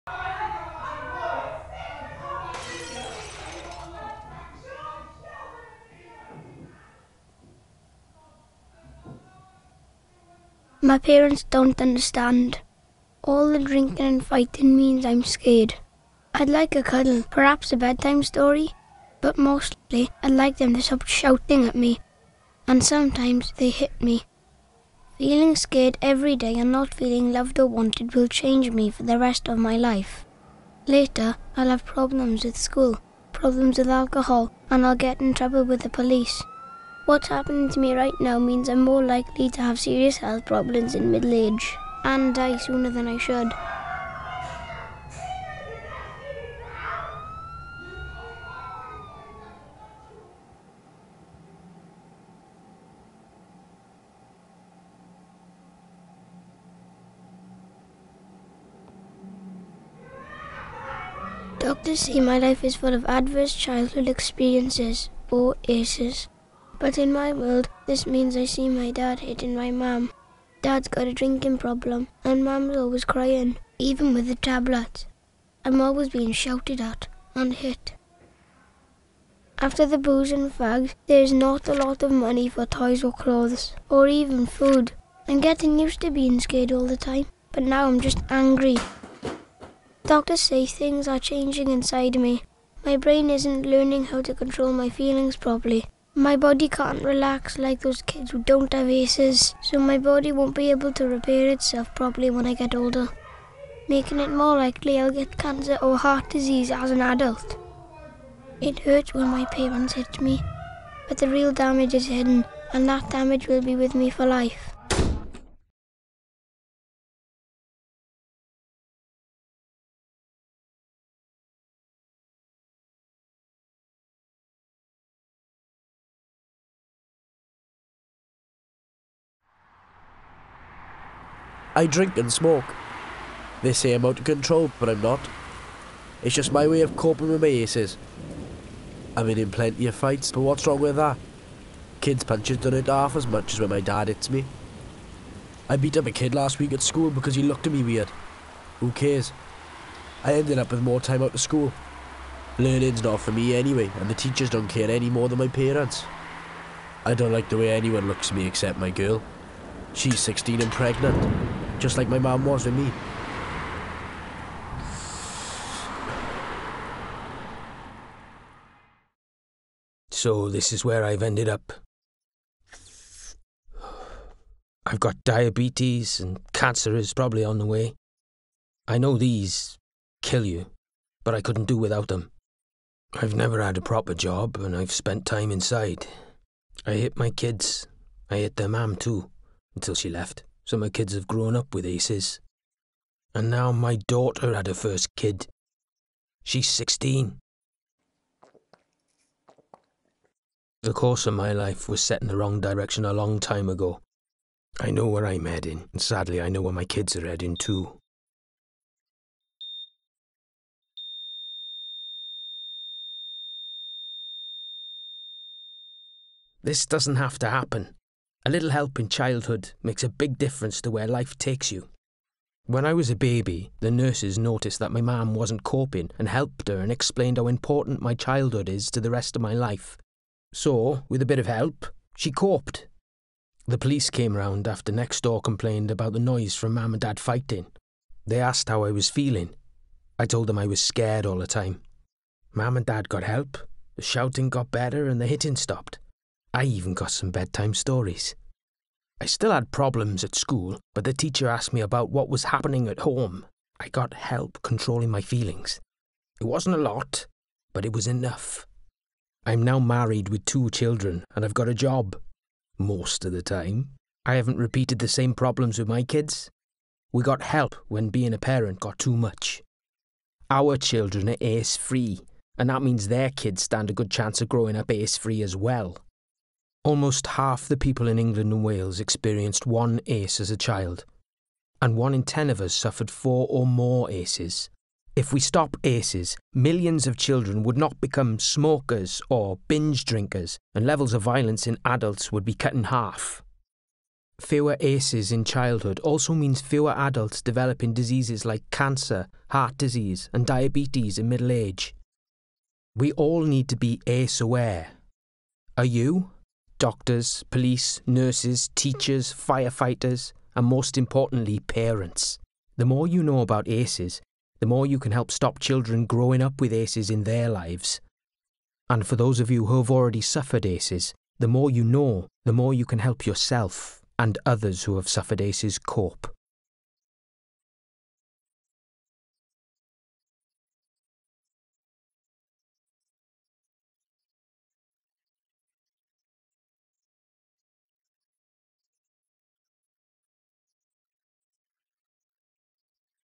Adverse Childhood Experiences (ACEs) (Wales) 2017
Adverse Childhood Experiences (ACEs) are traumatic events that affect children while growing up, such as suffering child maltreatment or living in a household affected by domestic violence, substance misuse or mental illness.

This short animated film has been developed to raise awareness of ACEs, their potential to damage health across the life course and the roles that different agencies can play in preventing ACEs and supporting those affected by them.

The film has been produced for Public Health Wales and Blackburn with Darwen Local Authority.